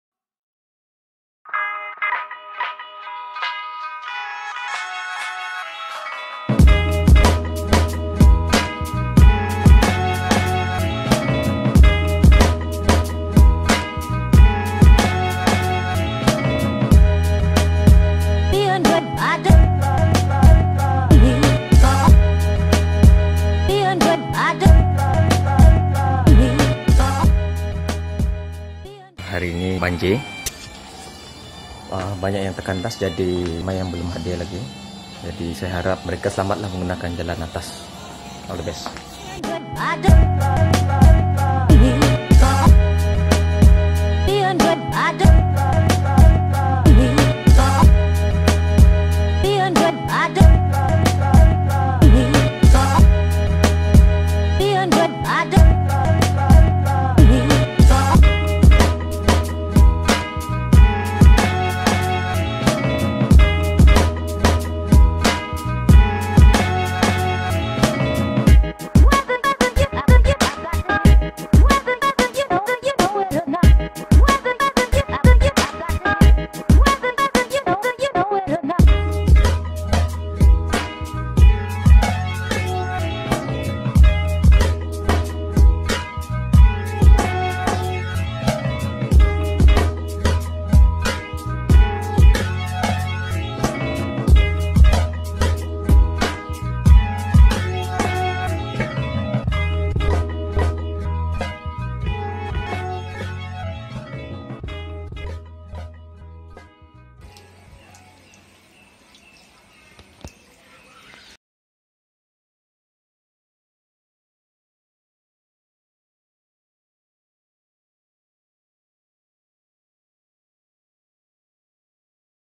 VLOG_046 : Drone Misi Meninjau Kawasan Banjir!!!
Shoot guna xiaomi redmi note 3 mount to koziro cinemamount mark ll, EKEN H3R.

DRONE : DJI PAHNTOM 3 STANDARD

EDITING SOFTWARE : Wondershare Filmora + VIDEOPAD